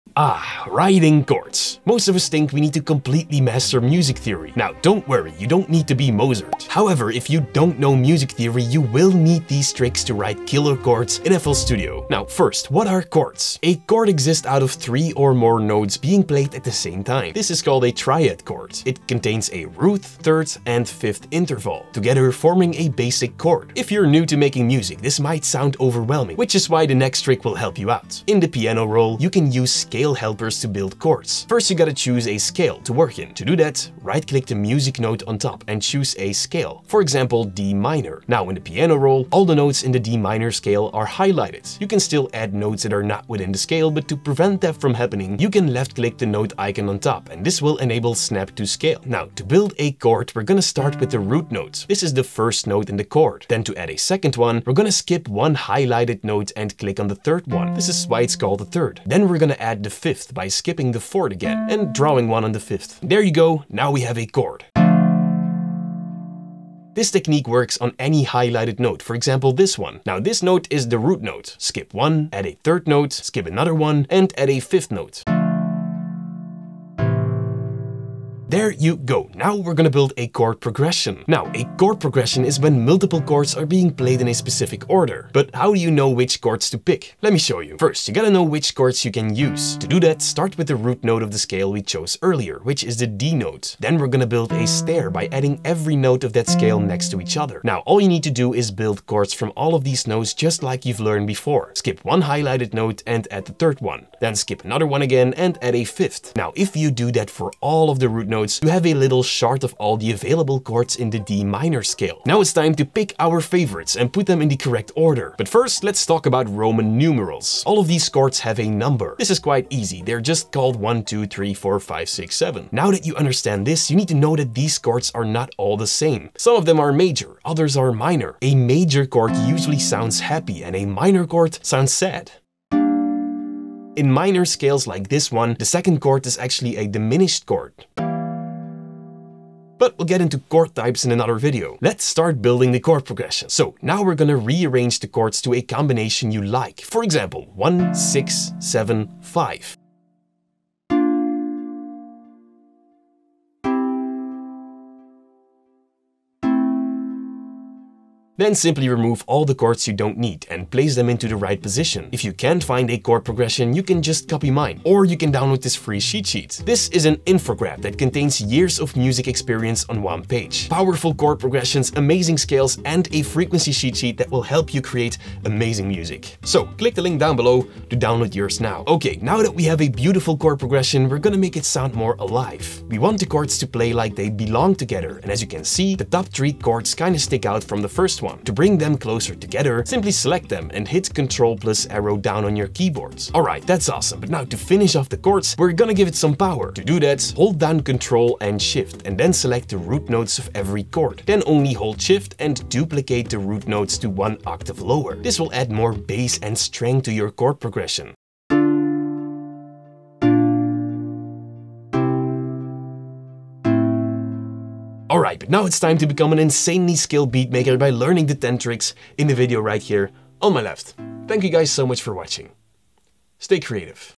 Write KILLER CHORDS For Beginners (FL Studio Tutorial)
👉 Get Your FREE 31+ Music Production Tips Infographic - Learn how to write killer chords and melodies for making beats for beginners in this FL Studio 21 tutorial video.